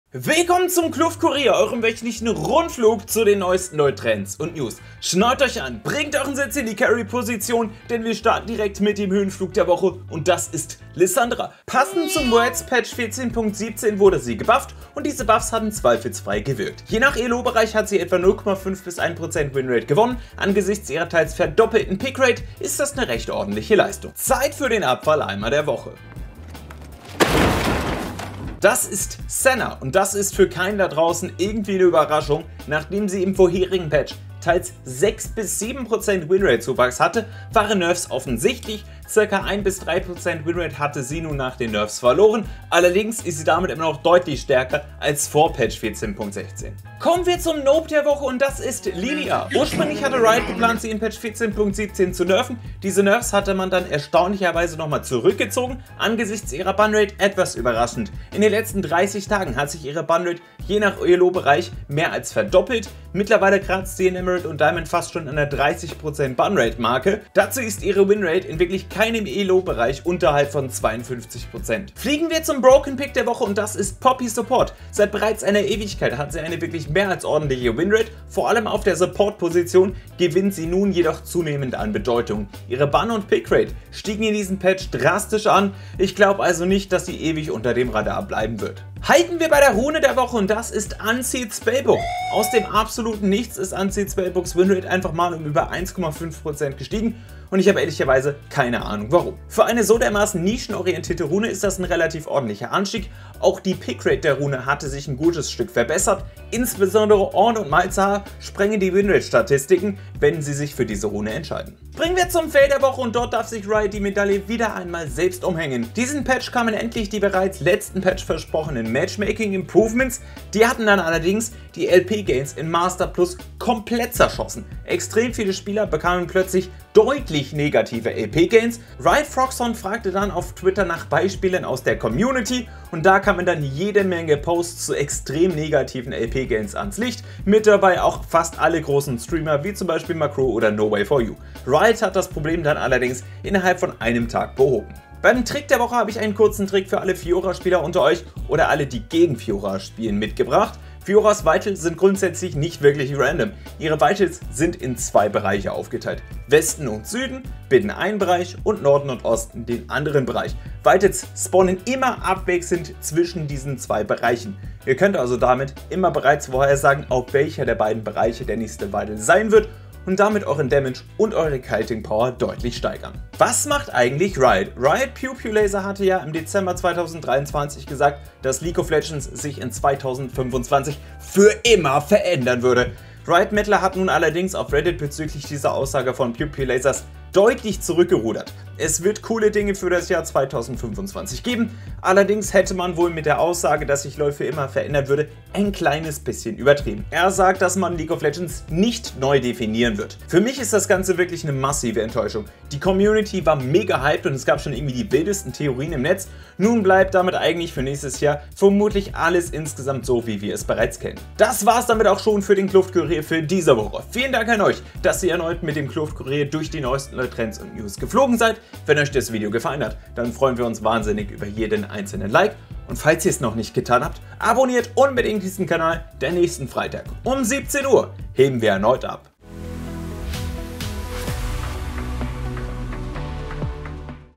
2025 verändert LoL FÜR IMMER! ODER? | Kluftkurier #31 | LoL News, Trends und mehr!
0:00 Intro
0:09 Höhenflug der Woche
0:24 Abfalleimer der Woche
0:47 NOPE der Woche
1:12 Broken Pick der Woche
1:29 Item der Woche
1:52 Fail der Woche
2:25 Trick der Woche
2:53 Was macht eigentlich Riot?
3:31 Outro

Das beste LoL News & Trends Format wäre ohne unsere fantastischen Sponsoren nicht möglich: